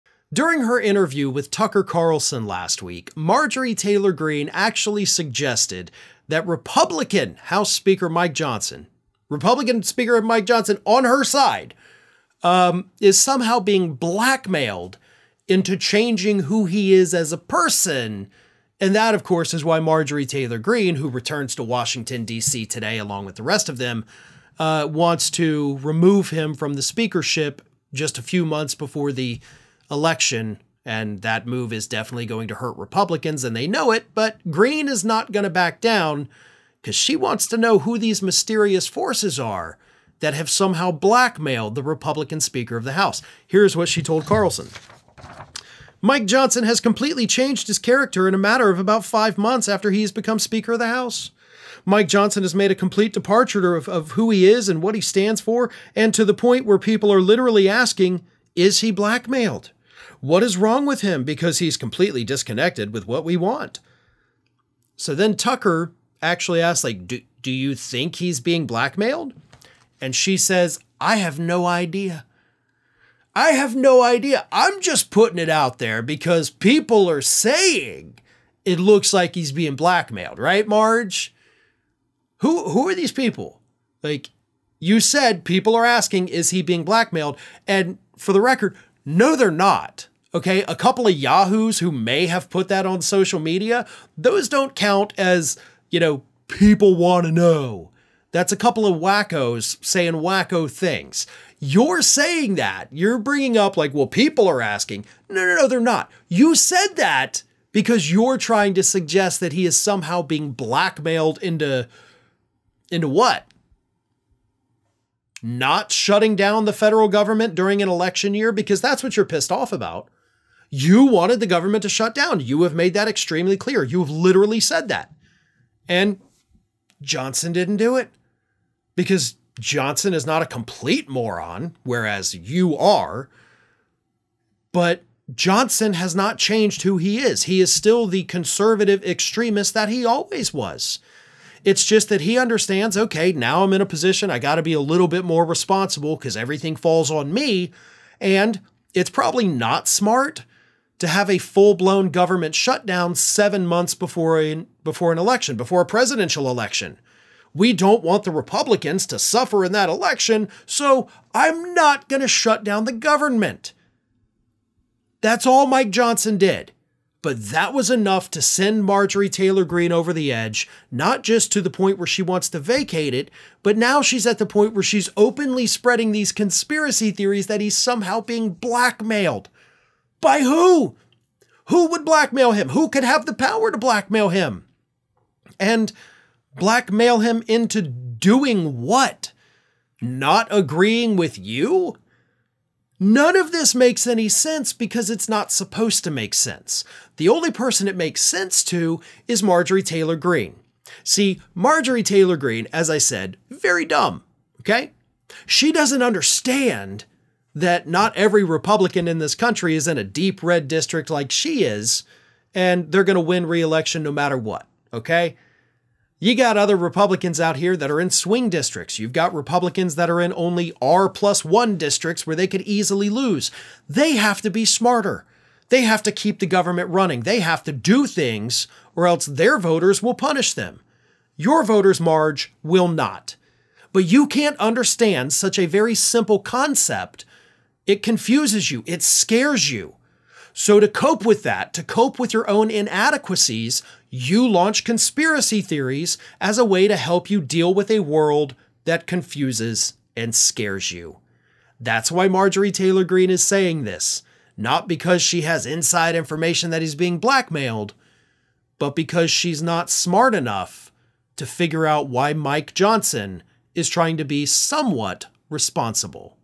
Marjorie Taylor Greene Suggests Mike Johnson Is Being Blackmailed By Mysterious Forces
During a recent interview with Tucker Carlson, Marjorie Taylor Greene said that Republican House Speaker Mike Johnson has "completely changed his character" in the five months since he has become Speaker, and she added that she doesn't know what is causing this change. This statement suggests that Greene believes there are mysterious forces at play that are causing Johnson to be a different person, which is how Greene handles a world that confuses and scares her. Farron Cousins explains what's happening.